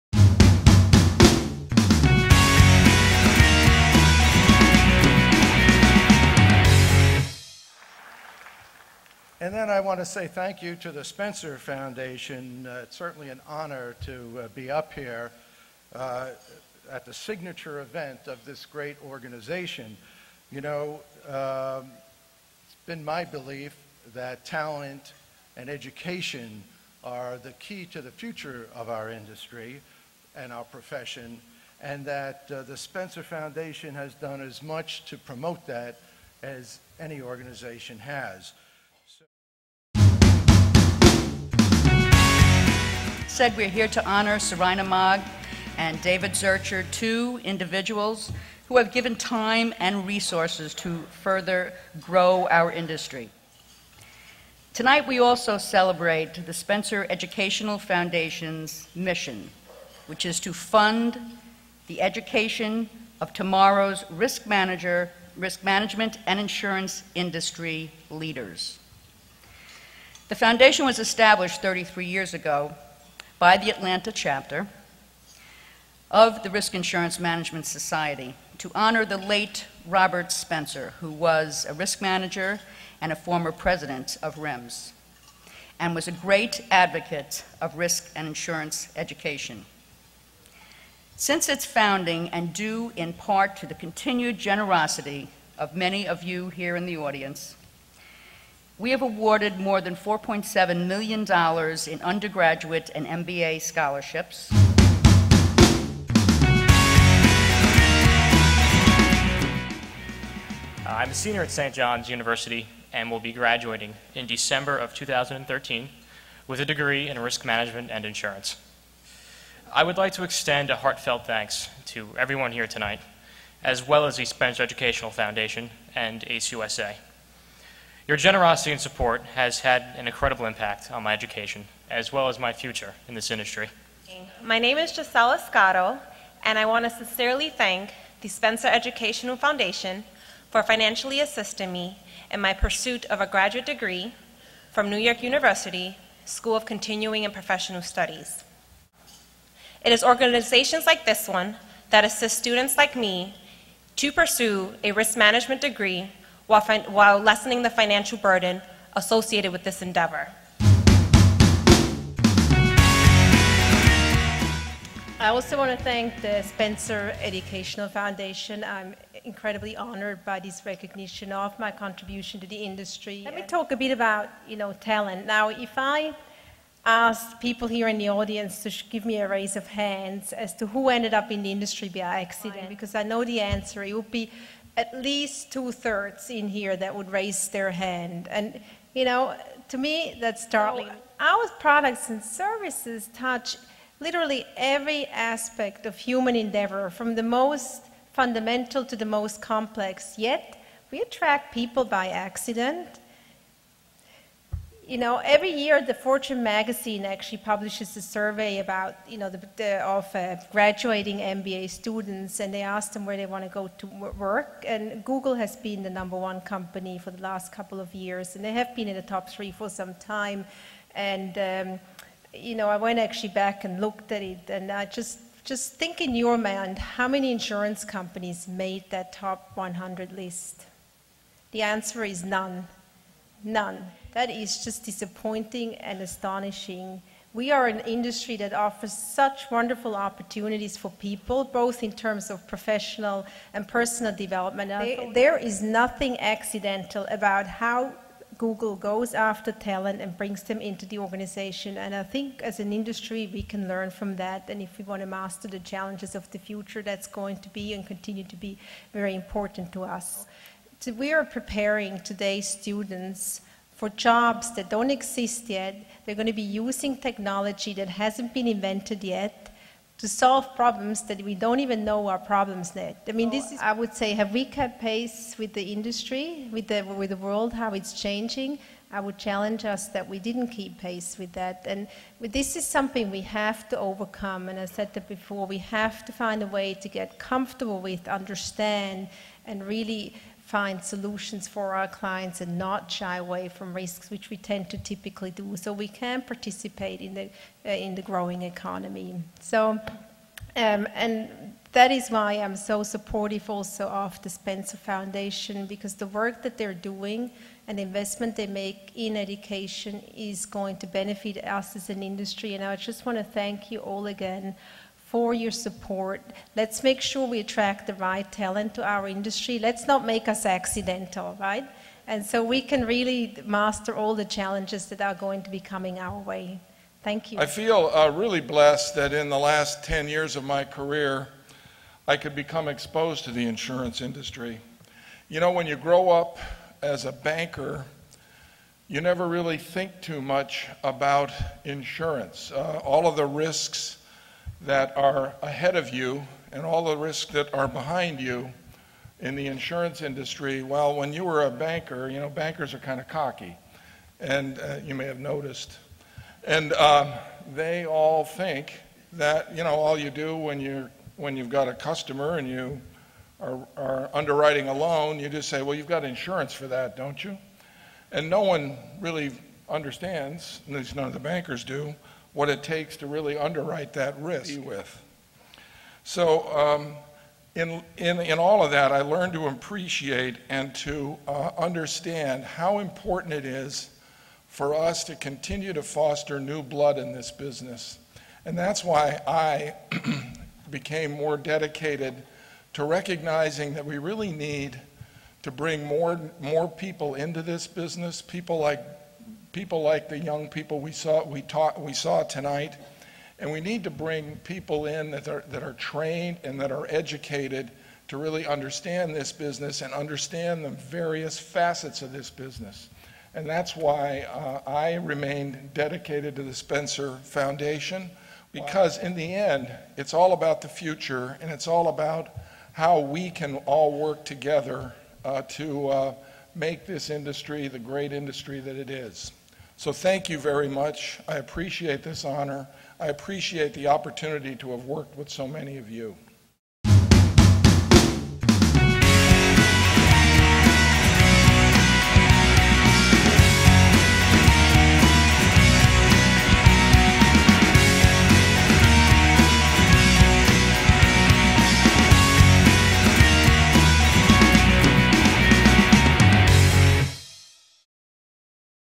2012 Spencer Educational Foundation Gala Dinner Highlights
The Spencer Educational Foundation, Inc. hosted its fourth annual fundraising gala on September 12, 2012, at the Waldorf-Astoria Hotel in New York City. This video provides highlights of the event.

During the event, the Foundation honored Seraina Maag, chief executive -- North America property and casualty for XL Insurance, and David Zuercher, retired head of insurance for Wells Fargo and Company, for their contributions to the advancement of risk management and insurance educational opportunities. Spencer Scholars Drew Carey and Gisselle Escotto spoke at the event. John Amore, CEO General Insurance of Zurich (retired), was the evening's master of ceremonies.